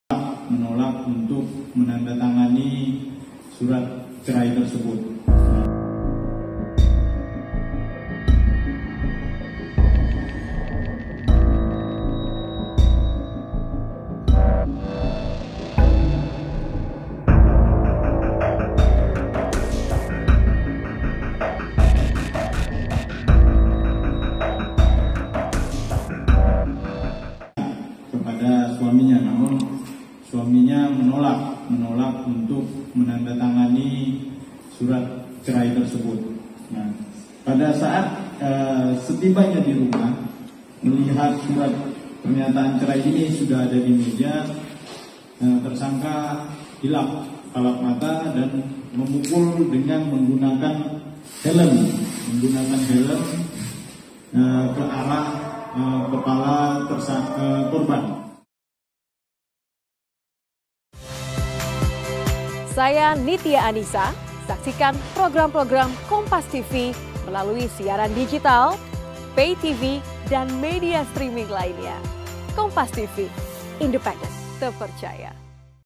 Menolak Diceraikan, Seorang Suami di Singkawang Tega Habisi Nyawa Istri!
SINGKAWANG, KOMPAS.TV   Pria tersebut berinisial BS yang tega membunuh istrinya sendiri dengan pisau dapur karena gelap mata. Diketahui BS emosi karena selalu didesak untuk menandatangani surat cerai.

Peristiwa nahas tersebut terjadi di rumah mereka tepatnya di Jalan Gunung Besi, Kelurahan Lirang, Kota Singkawang. Sebagai informasi, ketika korban tergeletak berlumuran darah tersangka langsung melarikan diri ke Polsek Singkawang Selatan, untuk menyerahkan diri.

Sebelum meninggal korban sempat dilarikan ke rumah sakit, namun sayang nyawanya tak tertolong.